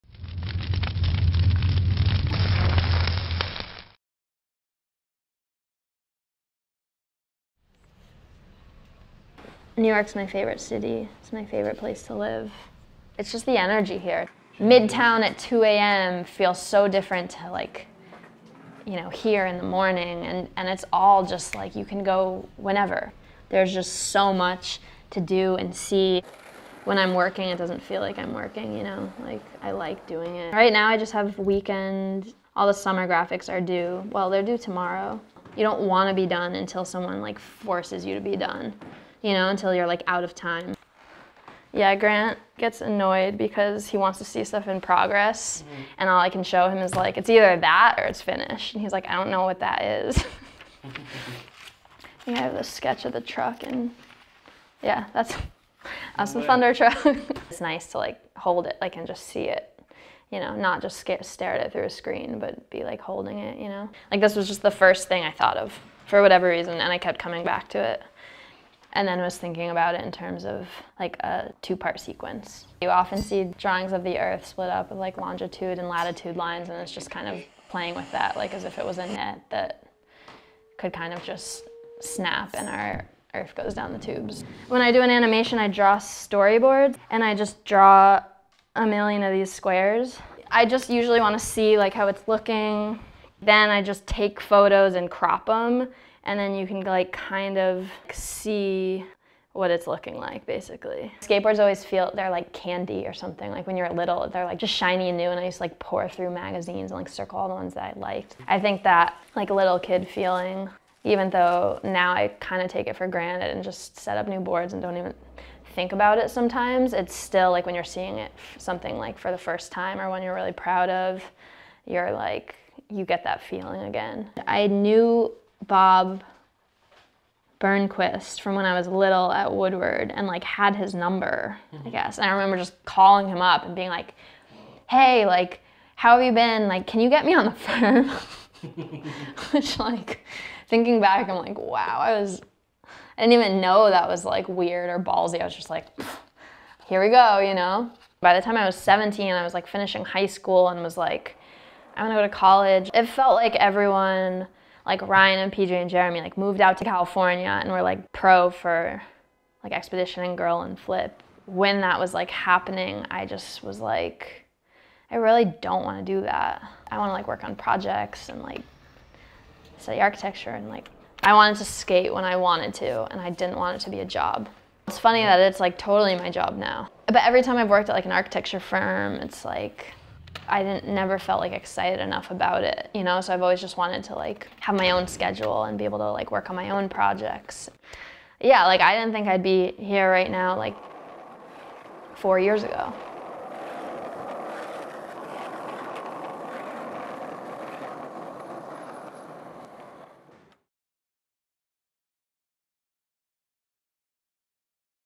Alexis Sablone's "Storyboard" Video
Between talking cold calls to Burnquist and the Coliseum days, Alexis cruises the streets of NYC, sharing her creative process designing a pro Thunder and showing appreciation for the city life. Big LOVE to Alexis and all of New York.